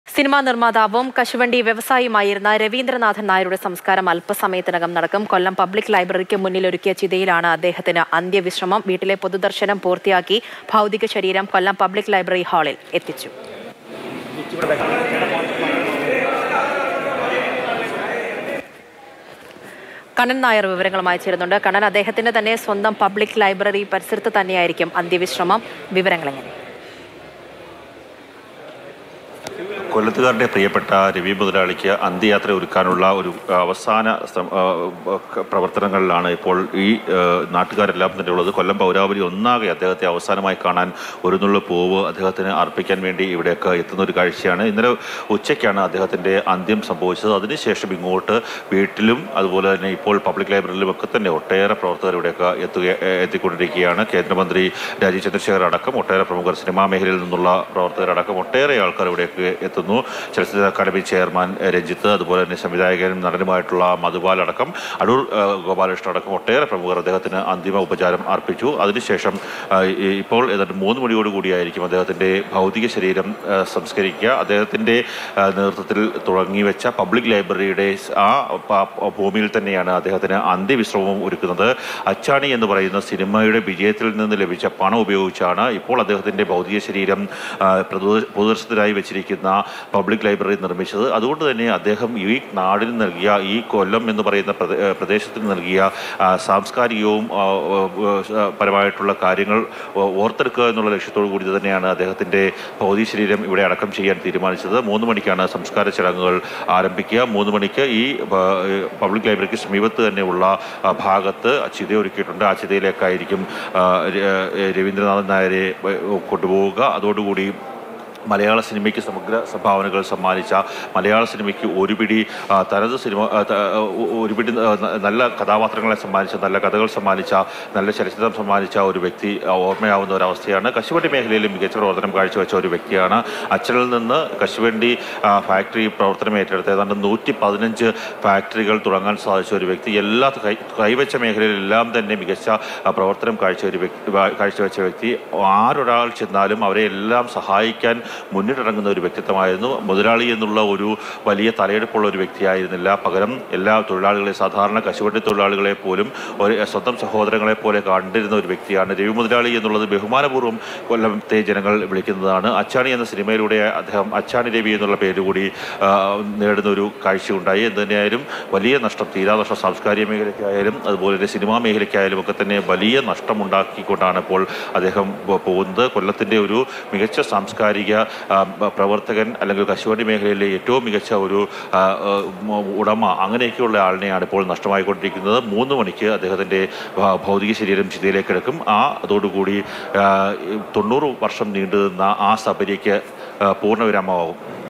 അച്ചാണി രവിക്ക് കൊല്ലം പബ്ലിക് ലൈബ്രറിക്ക് മുന്നിൽ ചിതയൊരുക്കും | Achani Ravi
സിനിമാനിർമ്മാതാവും കശുവണ്ടി വ്യവസായിയുമായിരുന്ന രവീന്ദ്രനാഥൻ നായരുടെ സംസ്കാരം അല്പസമയത്തിനകം. കൊല്ലം പബ്ലിക് ലൈബ്രറിക്ക് മുന്നിൽ ഒരുക്കിയ ചിതയിലാണ് അദ്ദേഹത്തിന് അന്ത്യവിശ്രമം.

Happy viewing!